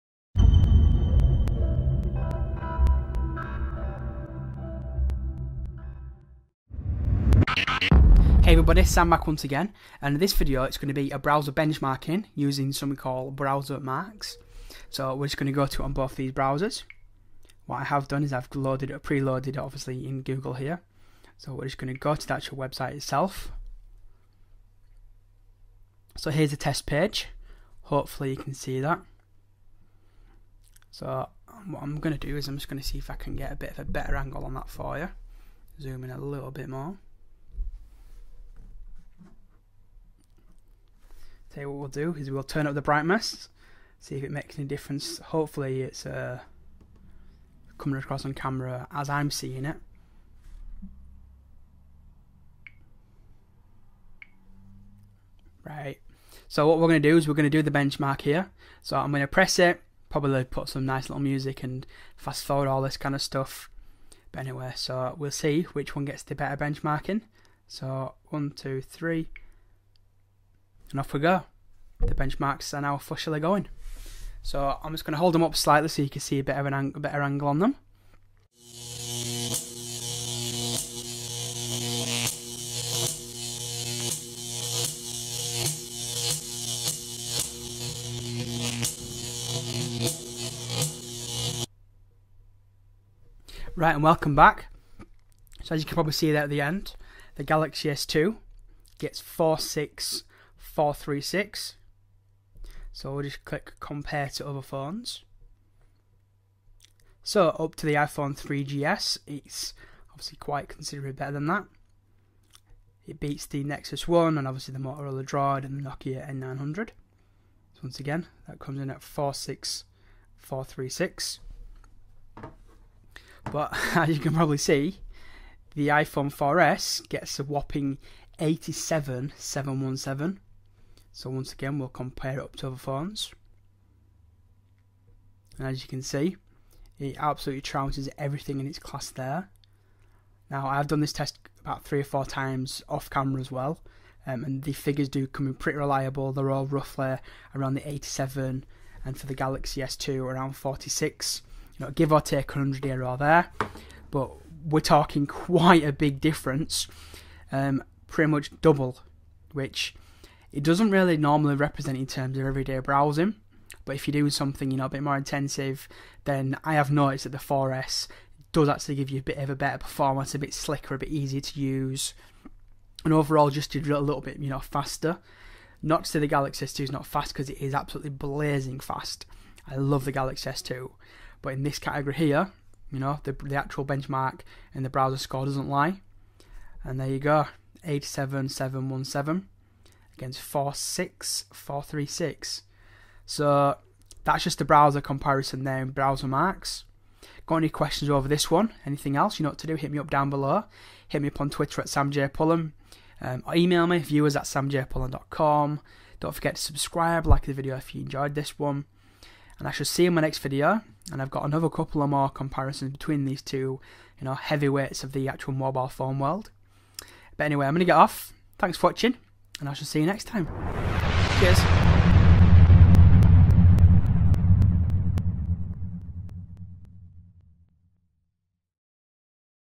Samsung Galaxy S2 vs iPhone 4S - BrowserMarks (Benchmarking)
Here is a browser benchmark - head to head with the 2 biggest titans on the mobile scene.. who will win ?